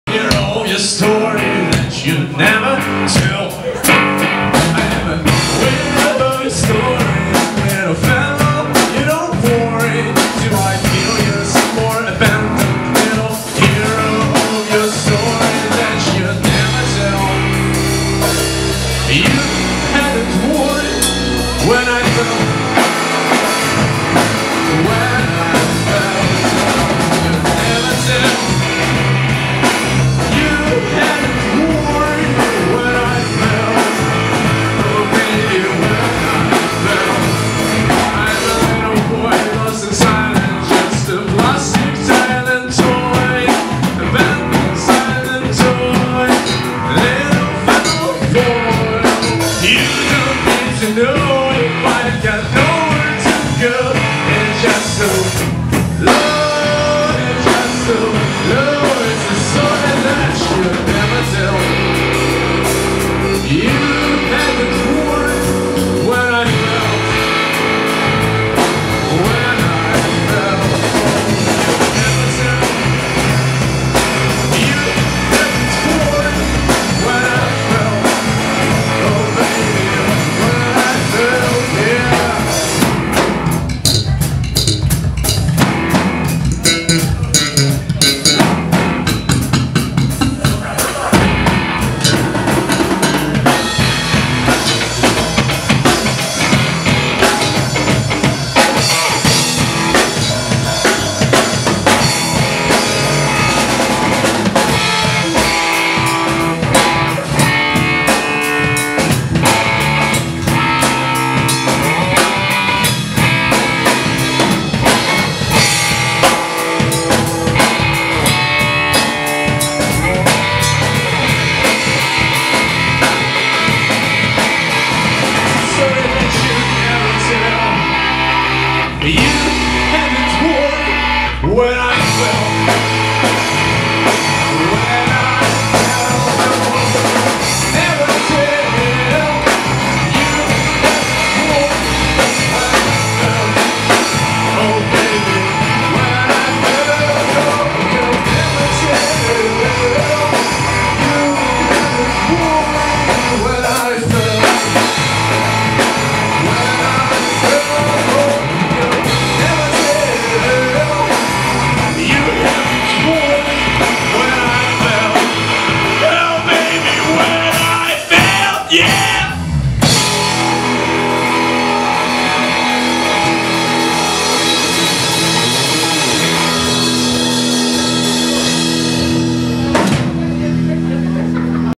Tiny Elephants Orchestra at Artishock bar (2)
Tiny Elephants Orchestra performs at Artishock bar.
30 August 2013. Bishkek, Kyrgyz Republic (Central Asia)